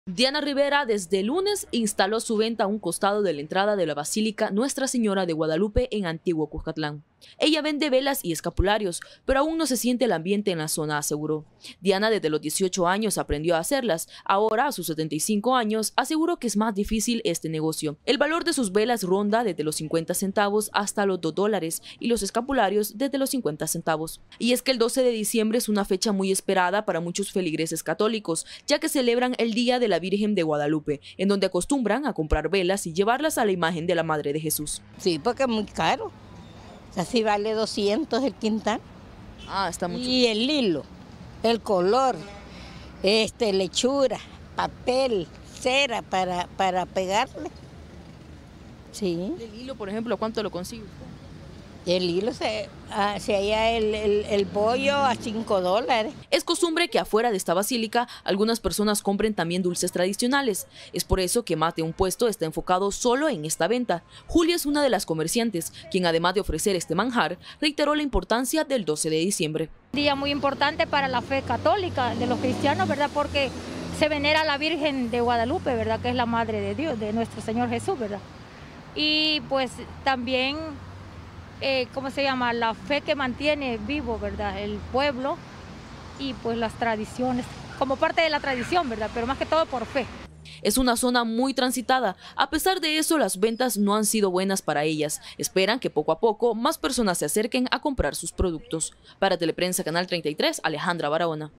Teleprensa33 | Vende escapularios y velas para el 12 de diciembre
Los comerciantes ya se están preparando para el 12 de diciembre, Día de la Virgen de Guadalupe.